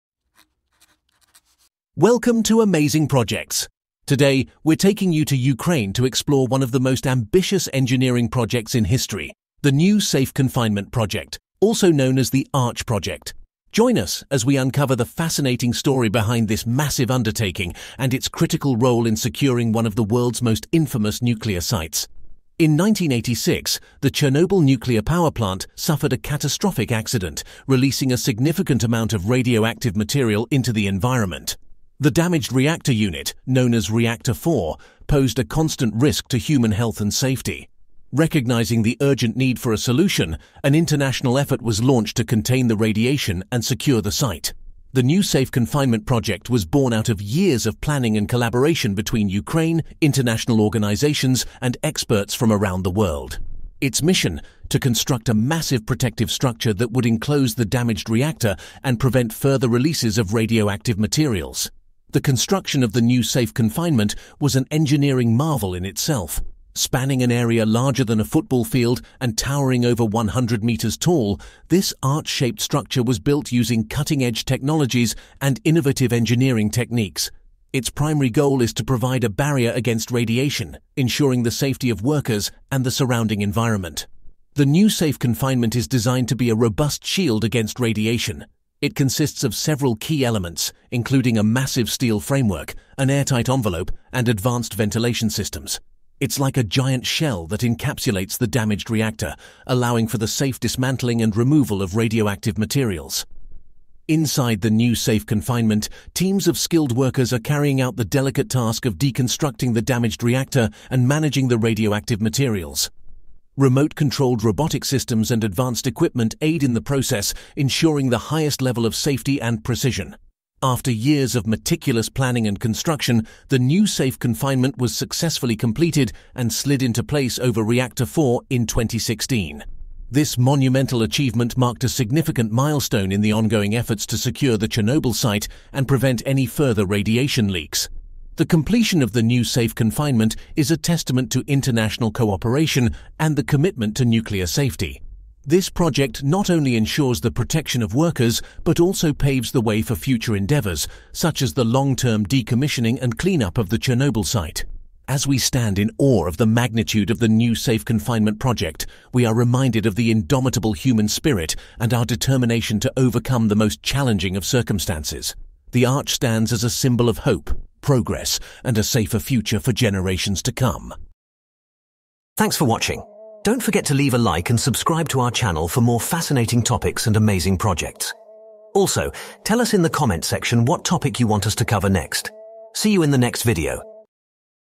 Keeping Ukraine Safe From The Chernobyl Radiation || The Sarcophagus
Welcome to Amazing Projects! Today, we're taking you to Ukraine to explore one of the most ambitious engineering projects in history—the New Safe Confinement project, also known as the Arch project. Join us as we uncover the fascinating story behind this massive undertaking and its critical role in securing one of the world's most infamous nuclear sites.
In 1986, the Chernobyl Nuclear Power Plant suffered a catastrophic accident, releasing a significant amount of radioactive material into the environment. The damaged reactor unit, known as Reactor 4, posed a constant risk to human health and safety. Recognizing the urgent need for a solution, an international effort was launched to contain the radiation and secure the site.
The New Safe Confinement project was born out of years of planning and collaboration between Ukraine, international organizations, and experts from around the world. Its mission: to construct a massive protective structure that would enclose the damaged reactor and prevent further releases of radioactive materials.
The construction of the New Safe Confinement was an engineering marvel in itself. Spanning an area larger than a football field and towering over 100 meters tall, this arch-shaped structure was built using cutting-edge technologies and innovative engineering techniques. Its primary goal is to provide a barrier against radiation, ensuring the safety of workers and the surrounding environment.